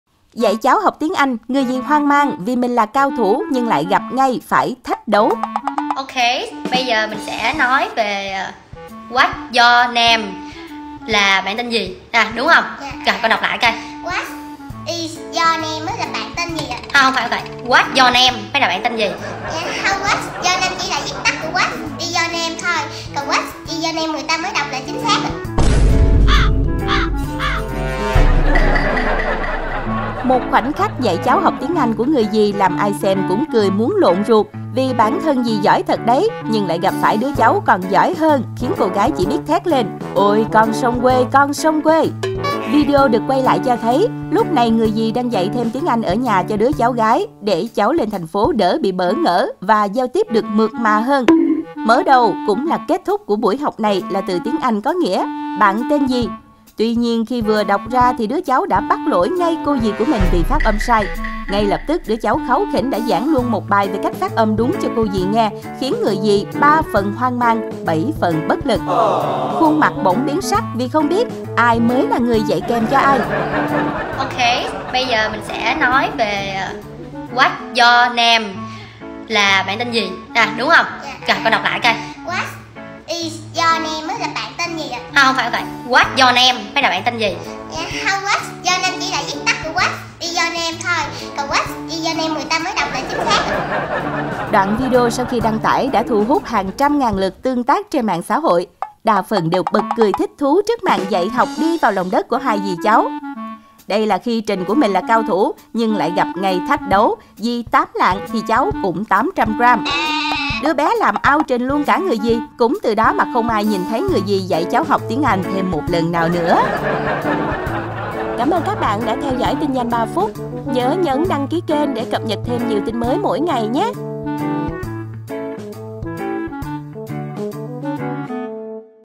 Dạy CHÁU HỌC tiếng anh, người dì HOANG MANG vì mình cao thủ lại gặp phải THÁCH ĐẤU |Tin Nhanh 3 Phút
Dạy 'CHÁU HỌC' tiếng anh, người dì 'HOANG MANG' vì mình là cao thủ nhưng lại gặp phải 'THÁCH ĐẤU' | Tin Nhanh 3 Phút